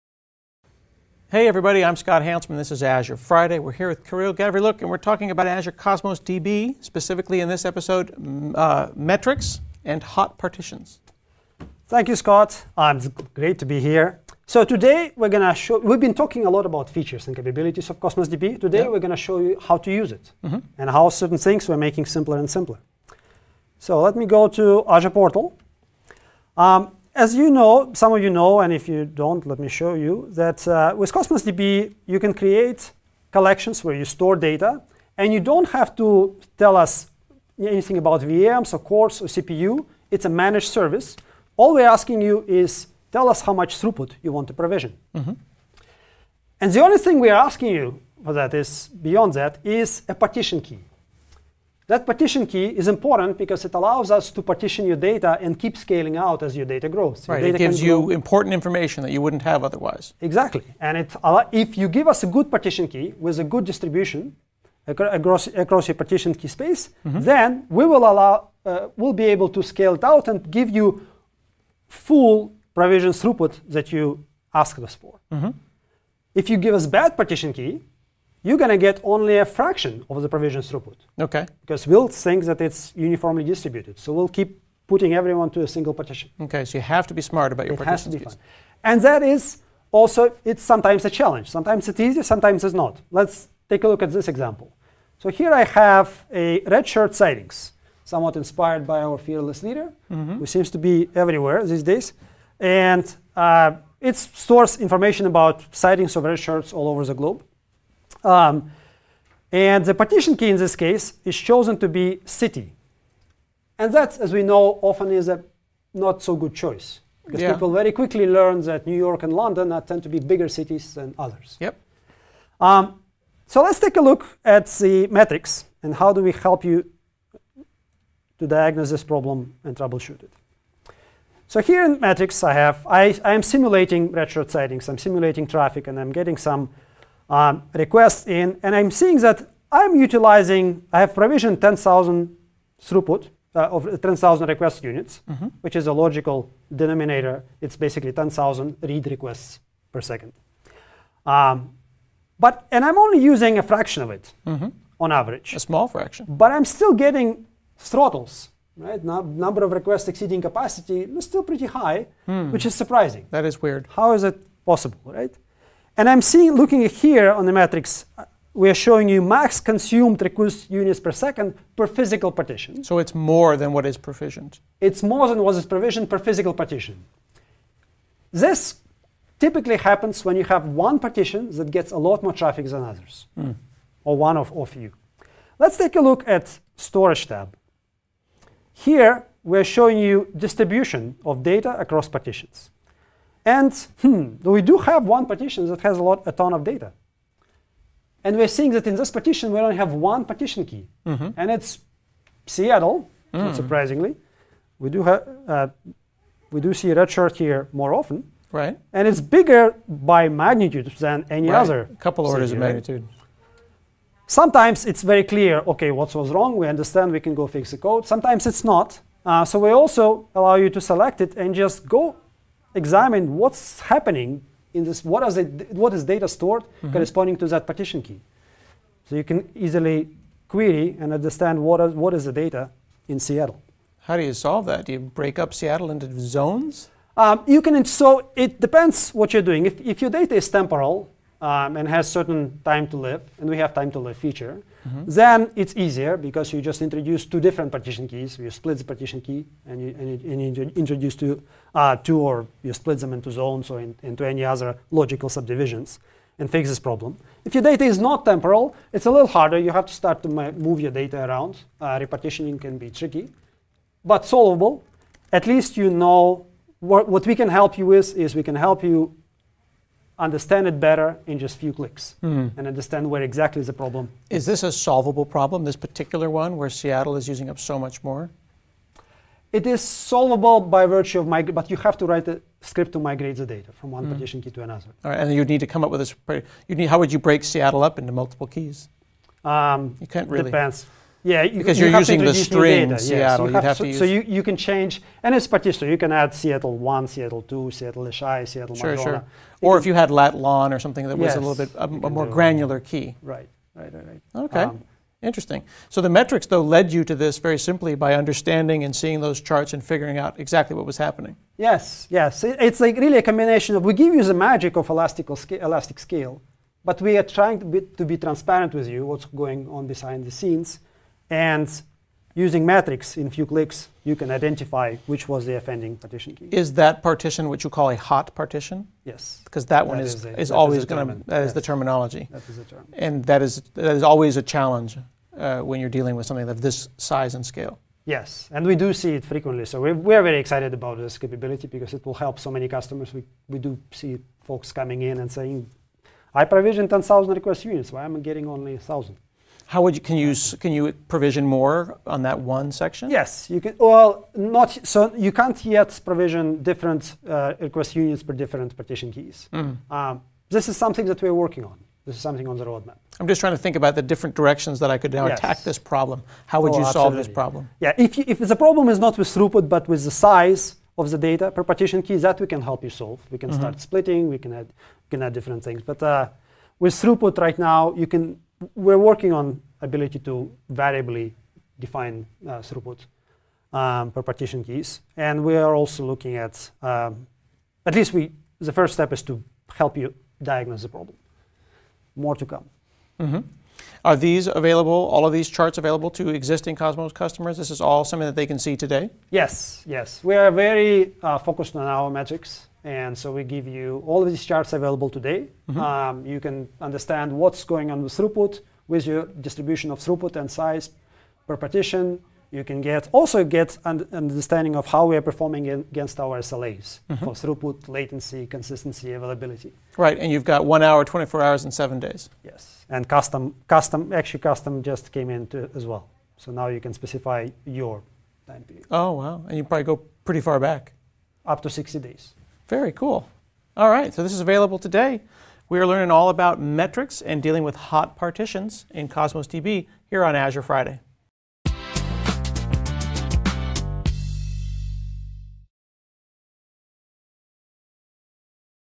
Azure Friday | Azure Cosmos DB: Get the Most Out of Provisioned Throughput
Azure Cosmos DB is a globally distributed database with limitless elastic scale. To take advantage of elastic scale, you first need to choose a partition key. Kirill Gavrylyuk stops by Azure Friday to talk with Scott Hanselman about the choice of partition key, and how to use the new metrics charts to troubleshoot a poor partition key choice (e.g., "hot partitions").